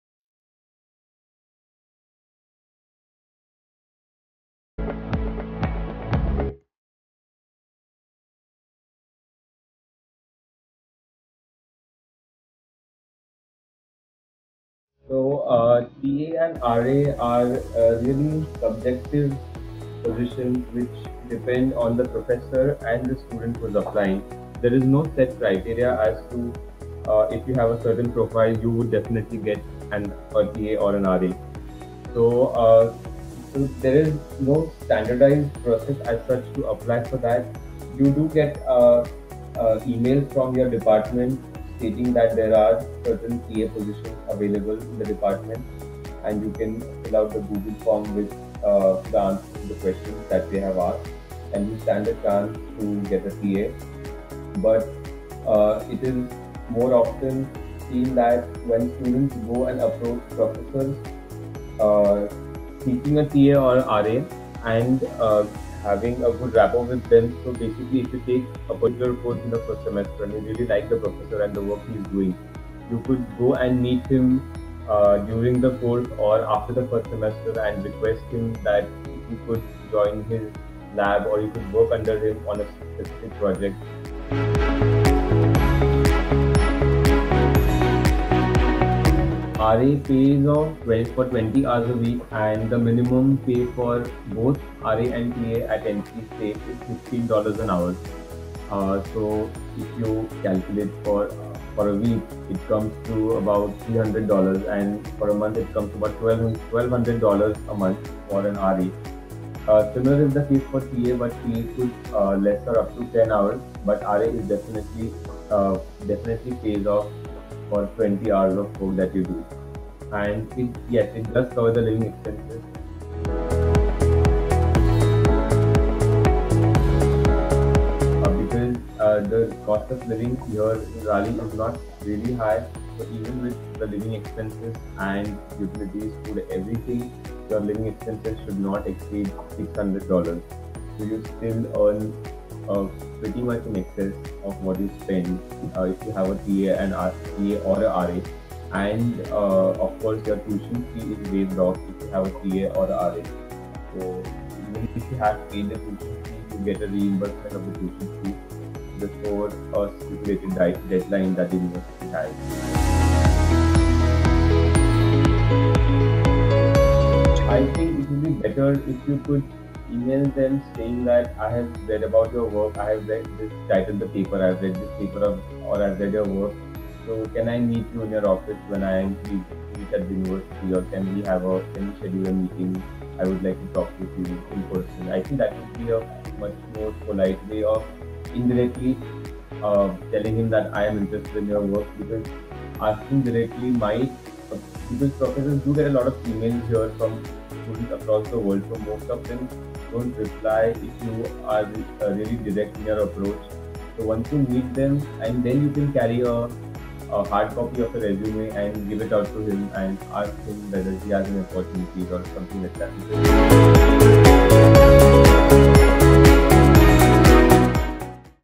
Exploring Financial Assistance at North Carolina State University
Meet Adhiraj, a Collegepond Alumnus and graduate of  North Carolina State University. He shares with you crucial firsthand information about financing your education at NCSU... right from TA/RA opportunities, on-campus jobs, earnings, initial expenses, scholarships opportunities, and more